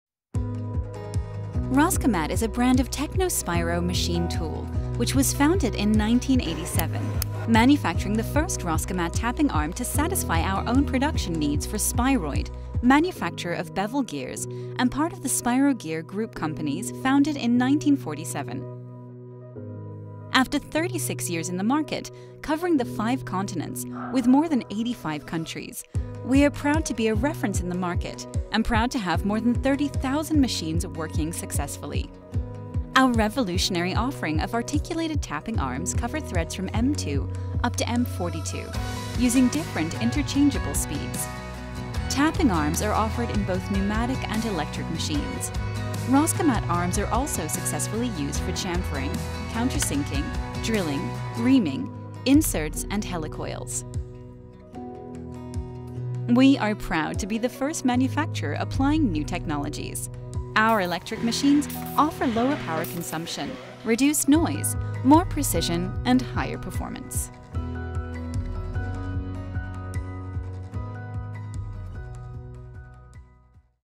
ROSCAMAT Company Overview
Roscamat is a brand of TECNOSPIRO MACHINE TOOL which was founded in 1987, manufacturing the first Roscamat tapping arm to satisfy our own production needs for Spiroide, Manufacturer of Bevel Gears and part of the Spirogear Group Companies founded in 1947.
After 36 years in the market, covering the five continents with more than 85 countries, we are proud to be reference in the market and proud to have more than 30,000 machines working successfully.
Our revolutionary offering of articulated tapping arms cover threads from M2 up to M42, using different interchangeable speeds. Tapping arms are offered in both pneumatic and electric machines. Roscamat arms are also successfully used for chamfering, countersinking, drilling, reaming, inserts and helicoils. We are proud to be the first manufacturer applying new technologies, our electric machines offer lower power consumption, reduced noise, more precision and higher performance.